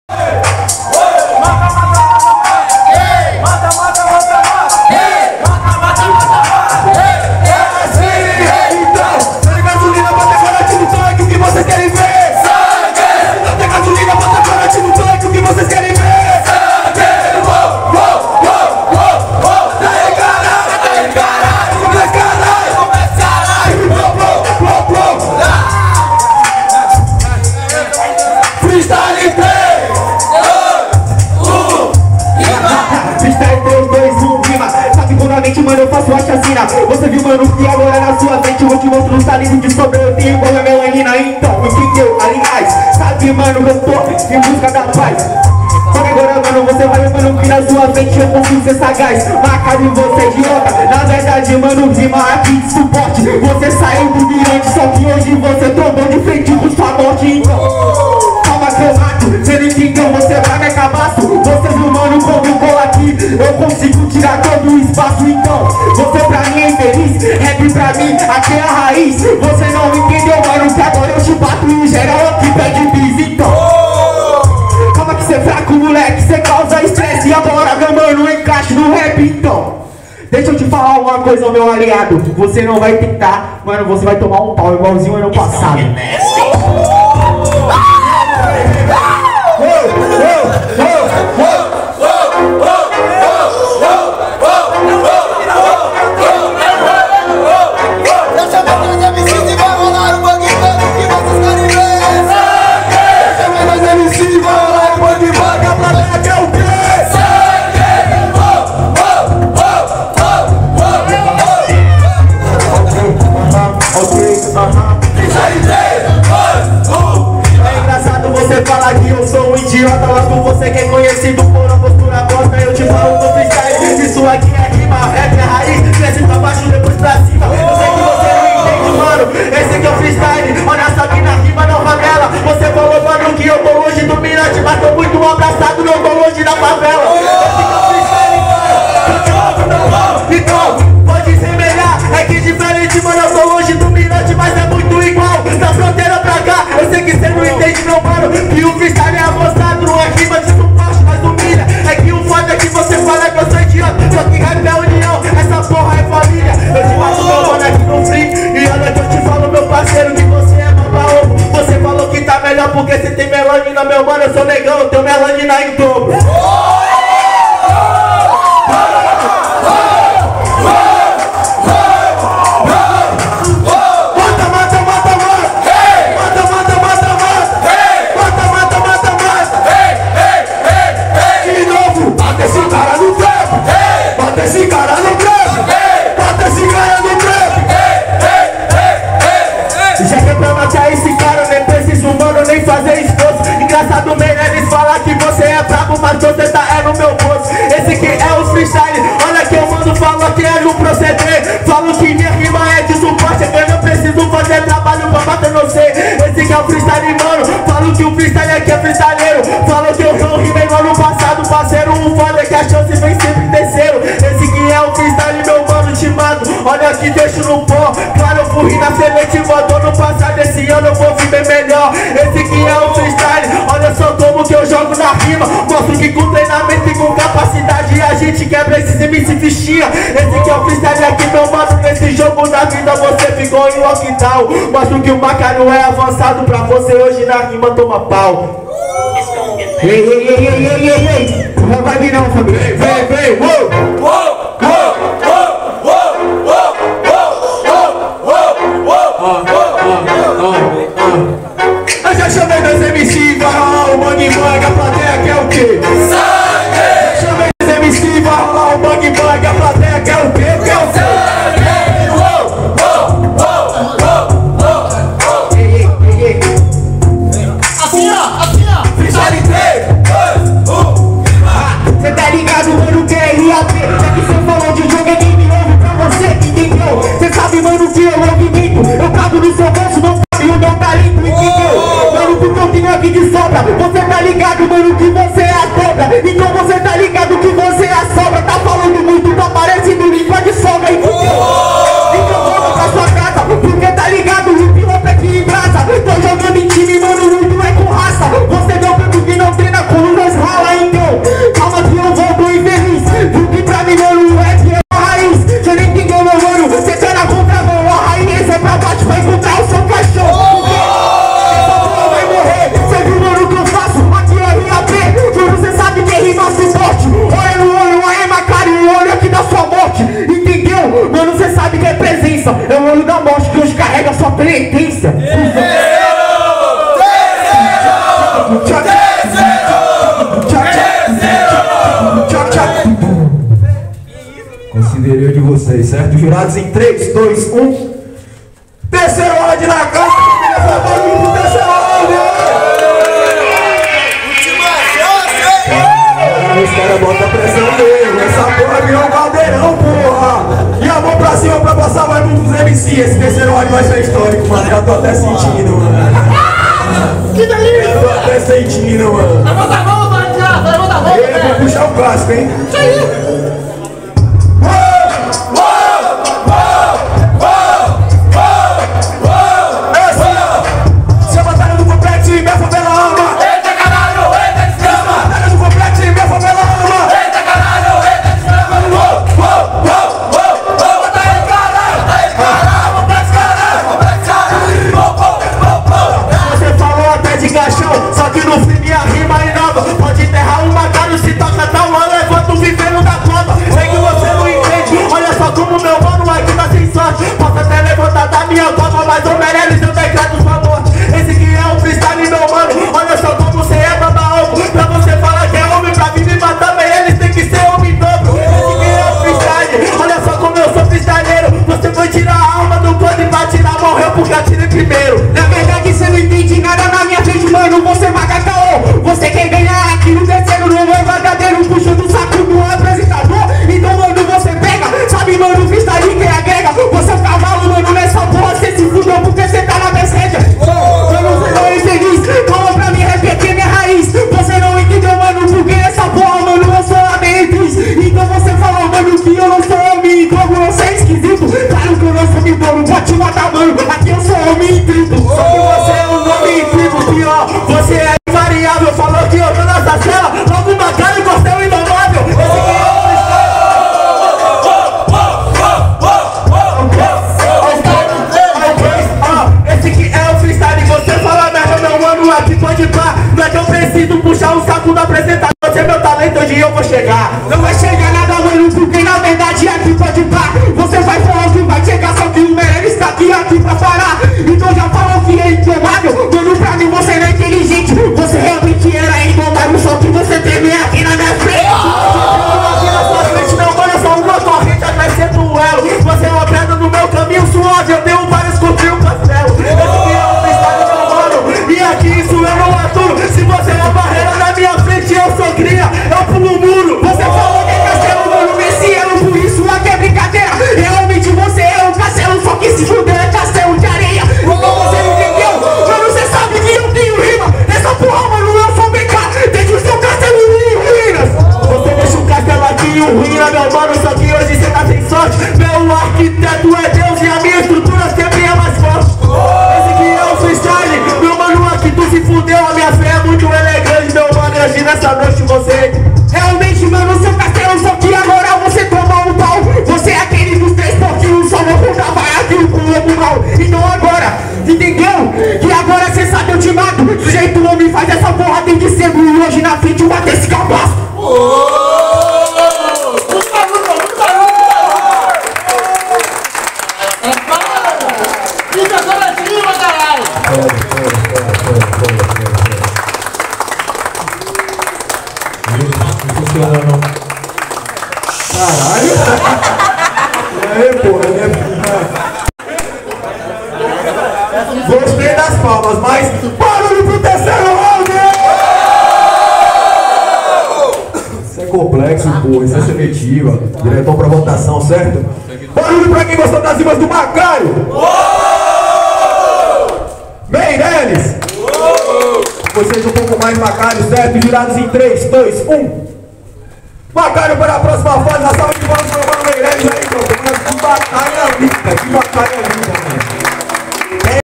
(🔥🔥🔥) MEIRELES X MACKARIO - PRIMEIRA FASE - 4° SELETIVA REGIONAL23 - EDIÇÃO COMPLEXO - BA
A Batalha do complexo é um movimento cultural da cidade de Porto Seguro -Ba. Movimento que vem solidificando cada semana com freestyle estilo livre na praça do bairro Frei Calixto. Batalha que vem ocupando espaços em escolas e universidade da cidade!

BATALHA TODO DOMINGO NA PRAÇA DO TRABALHADOR - BAIANÃO (PORTO SEGURO-BA)